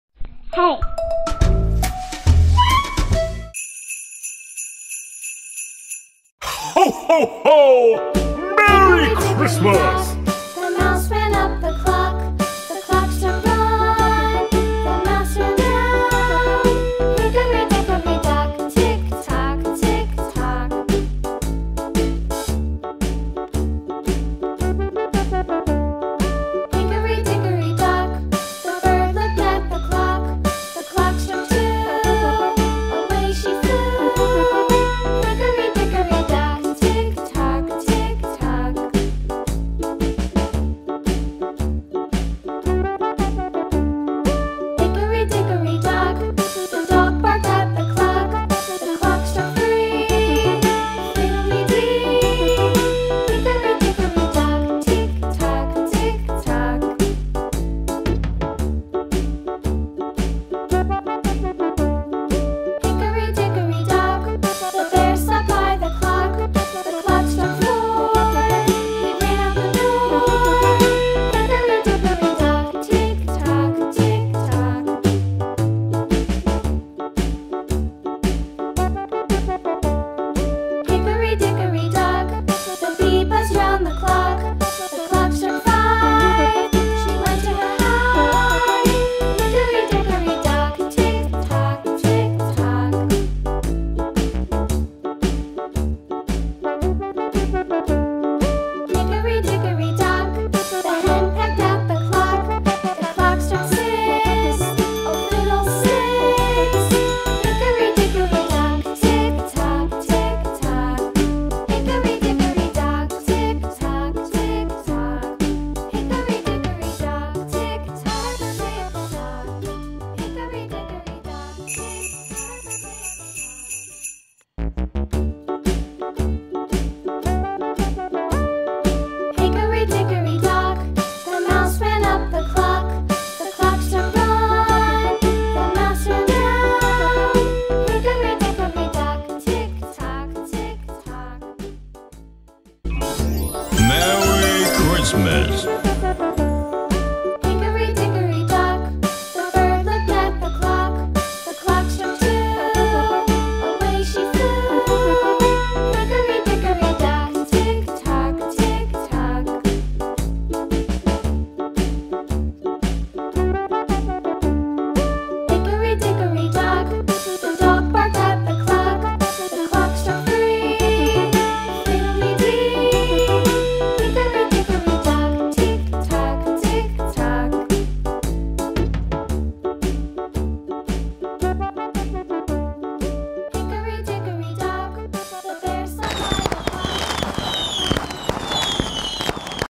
Drawing Mickey Mouse is ready to decorate the Christmas tree for Christmas.
Mickey Mouse is ready to decorate the Christmas tree for Christmas.